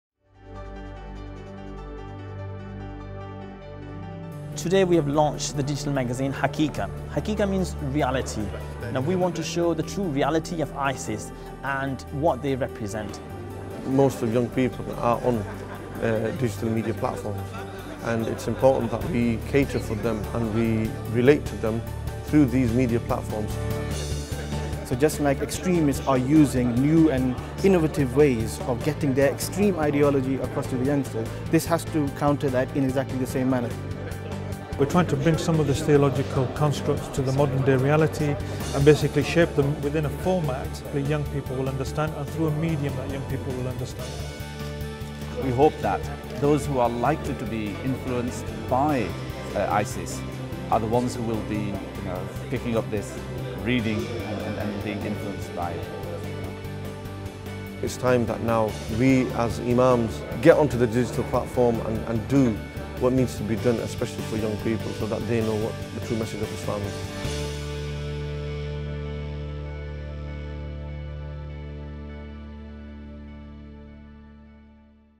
"Haqiqah" - What is the truth behind ISIS? Digital Magazine Launch
Watch this video higlighting the launch of "Haqiqah" - a digitial magazine exposing the reality of ISIL. The Magazine focuses on providing the correct understanding of Islam using Qur'an and authentic Hadith. Imams and Scholars explain the importance of reaching out to the youth in innovative ways.